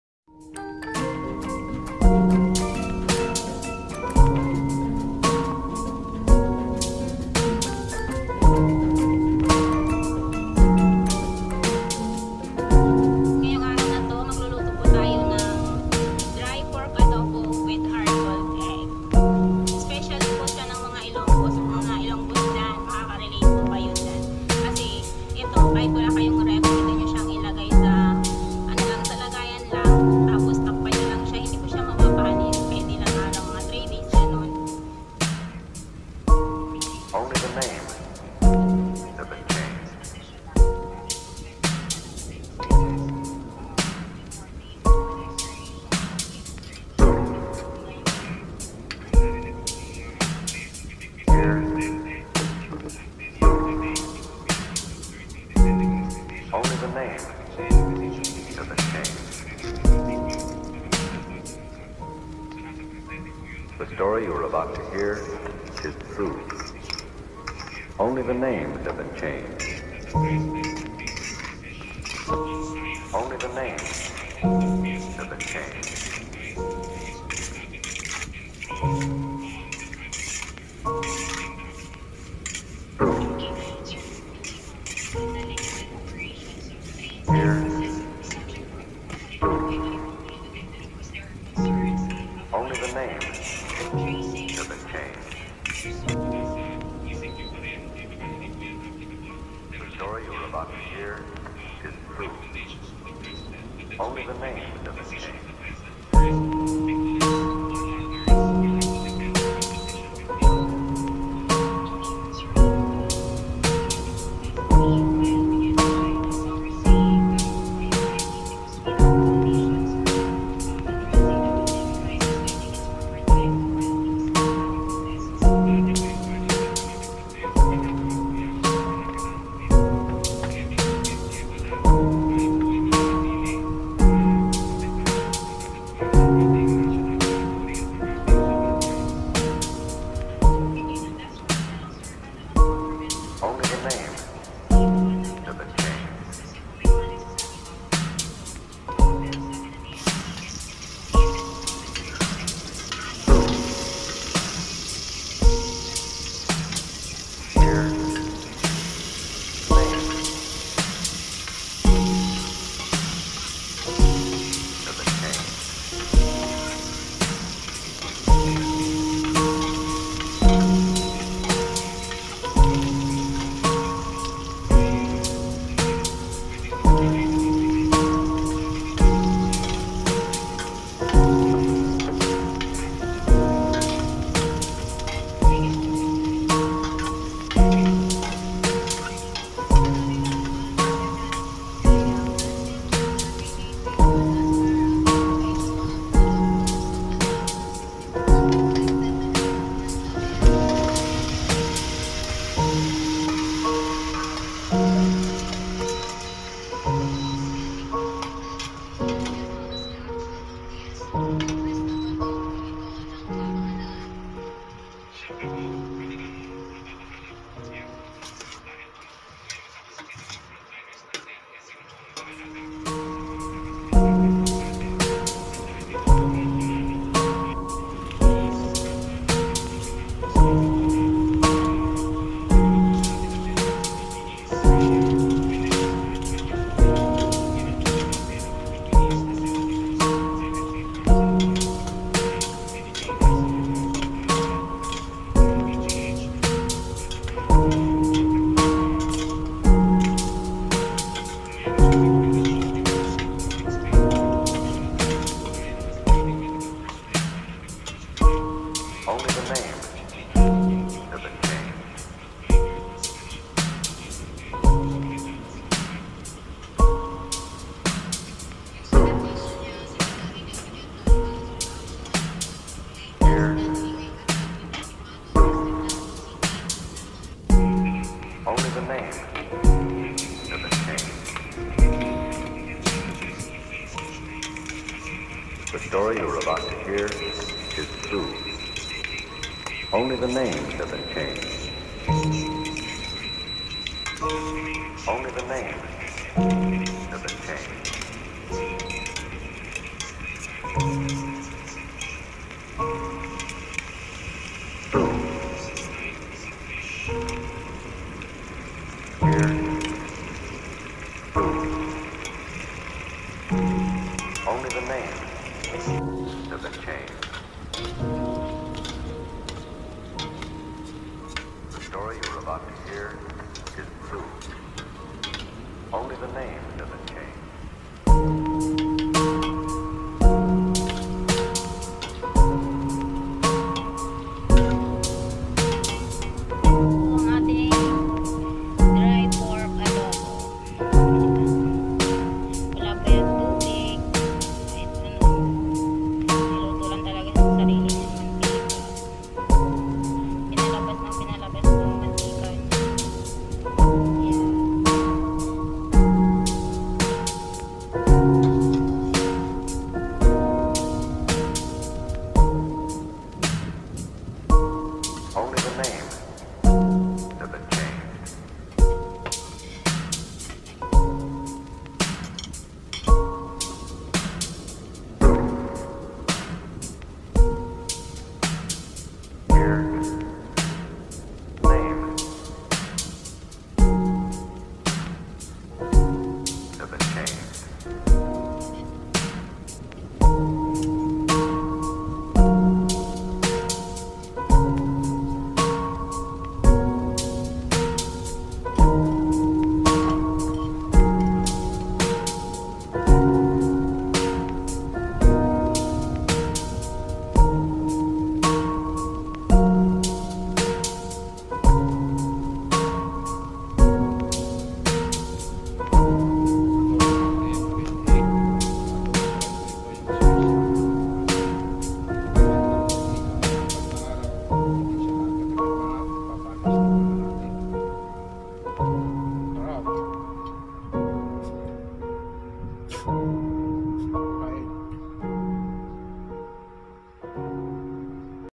HOW TO COOK ILONGGO'S BEST PORK ADOBO
HowtoCookIlonggoesBestPorkAdobo pork adobo
Hello mga kaLAFANG k jan.Our best seller Ilonggo's best dry pork adobo w/hard boiled egg.Ang advantage nito..it last up to 3days even w/out putting inside the friged..
INGREDIENTS:
PORK Kasim
Soy sauce
Vinegar
Brown sugar or honey
Oil
Onion
Garlic
Powder/whole black pepper
Bay leaves